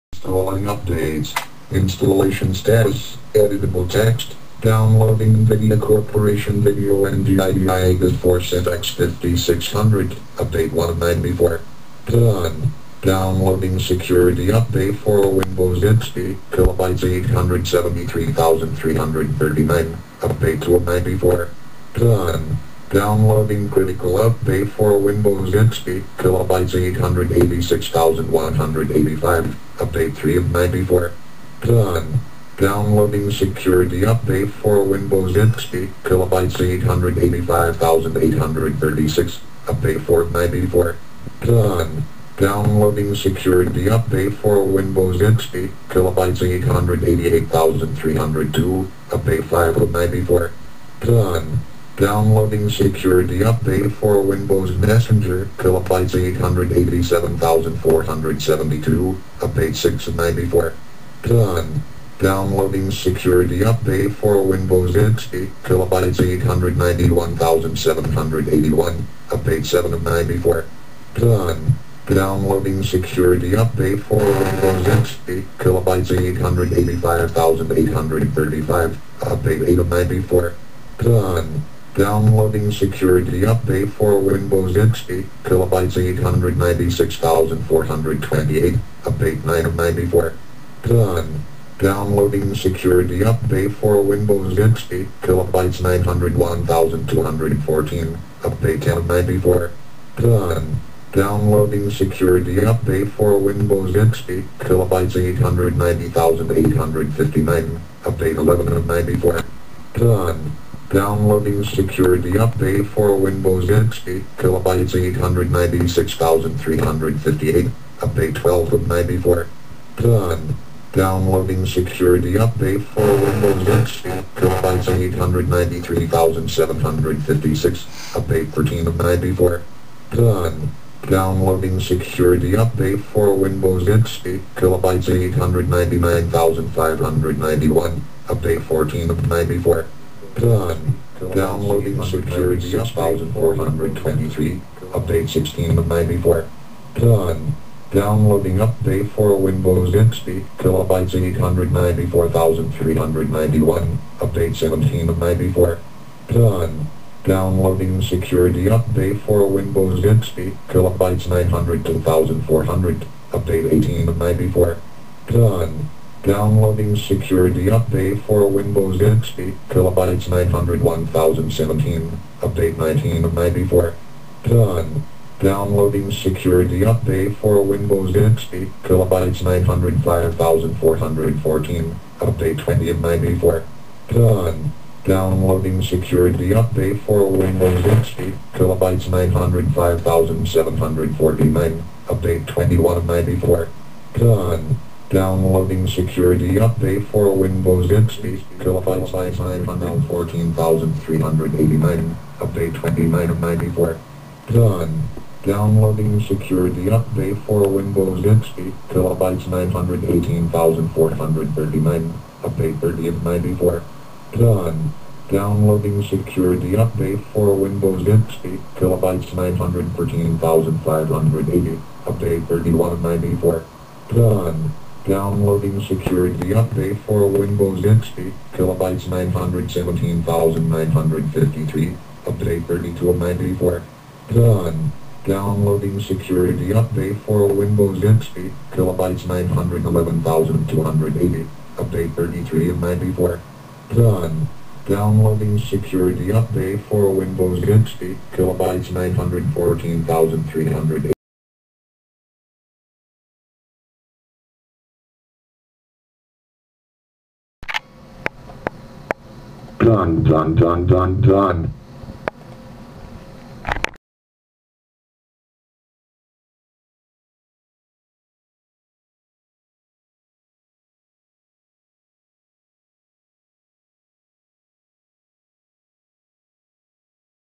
Updates with MS Sam
expand to read the WHOLE THING
Have u noticed that the majority of these updates are security updates?!? lol! listen to ms sam read aloud all 94 of my updates